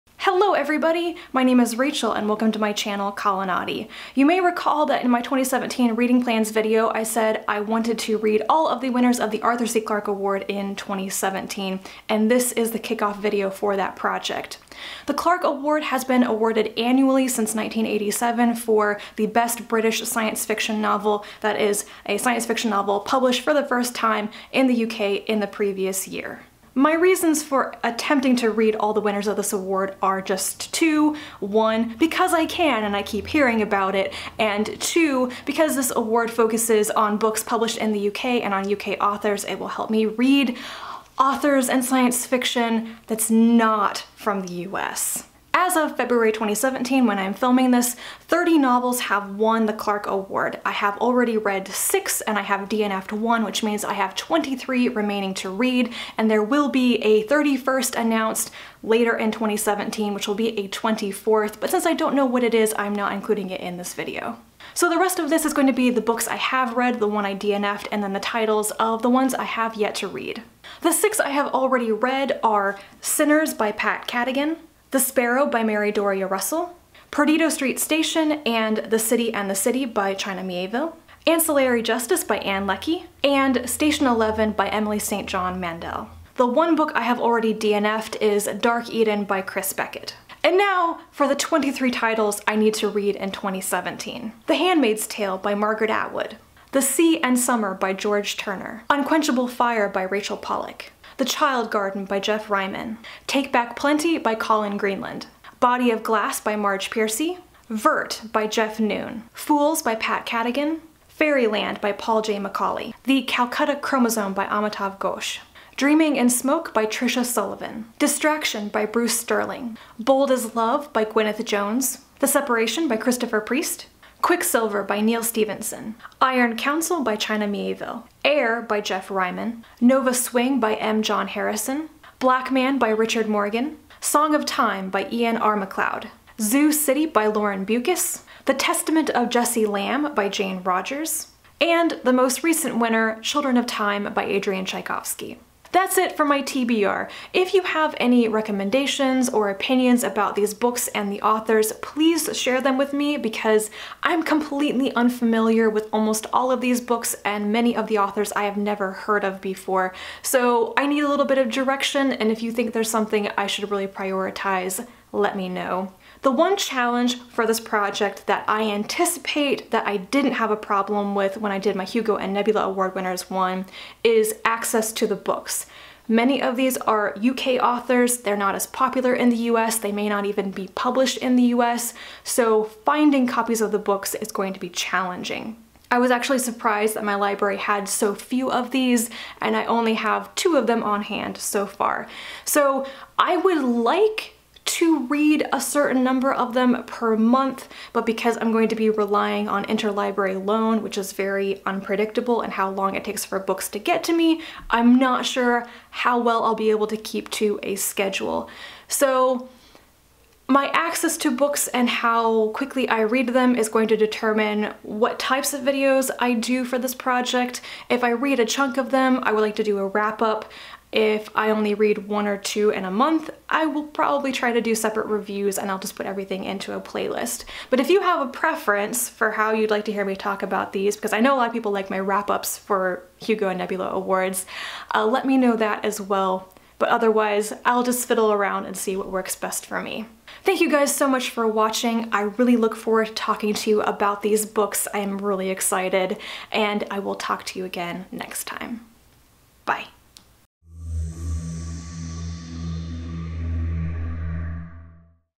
Reading the Arthur C. Clarke Award Winners!
It's time to tackle my Clarke Award project for 2017! It's going to be much less intense than the Hugo/Nebula project, for sure.

(*Note: As a few kind people noted in the comments, a couple of these books were published under different titles in the U.S.

Body of Glass = He, She, and It
Black Man = Thirteen
The Sea and Summer = Drowning Towers)